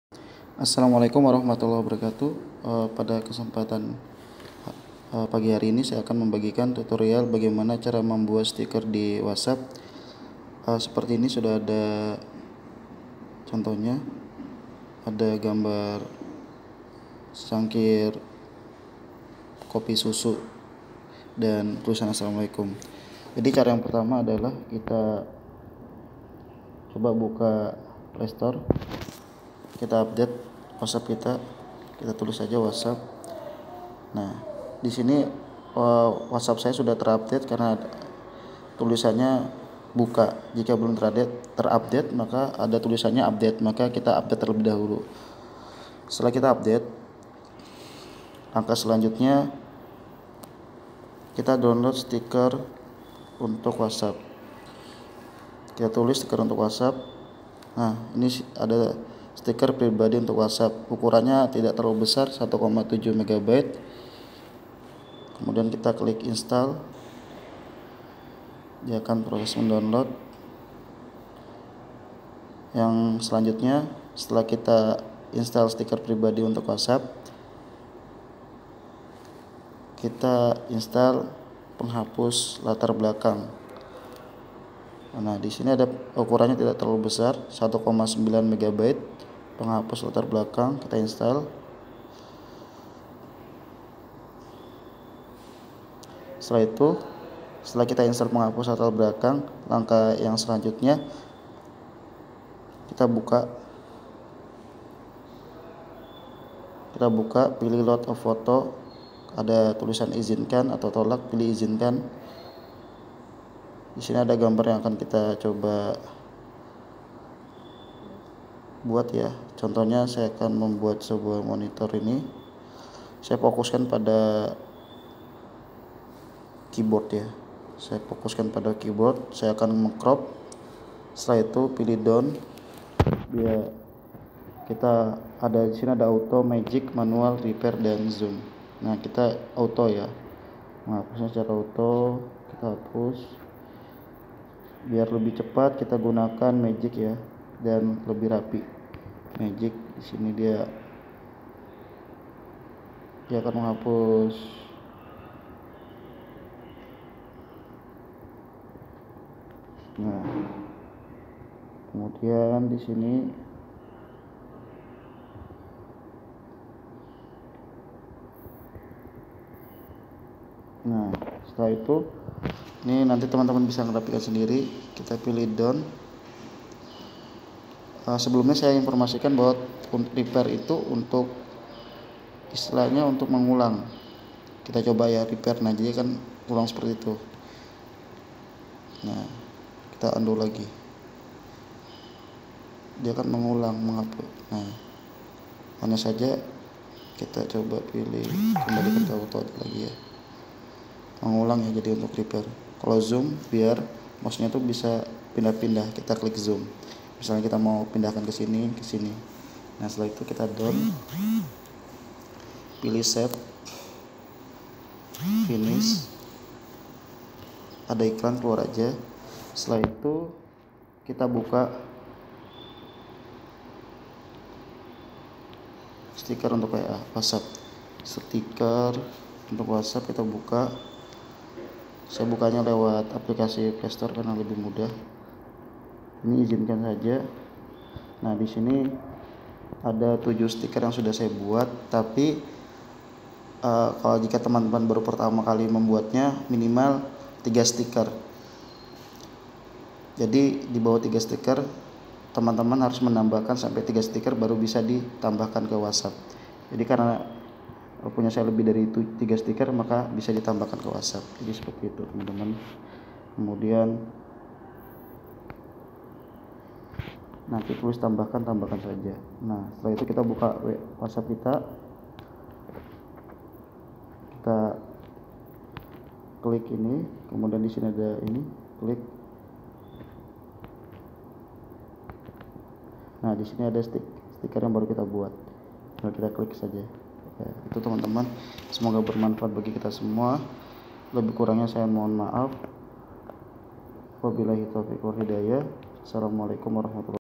Tips membuat Stiker di whatshapp yang unik dan meyenangkan
Tips untuk membuat sendiri stiker di whatshapp :
1. Update whatshapp
2. Download di playstore Stiker Pribadi untuk whatshapp
3. Download di playstore Penghapus latar belakang

Semoga bermanfaat..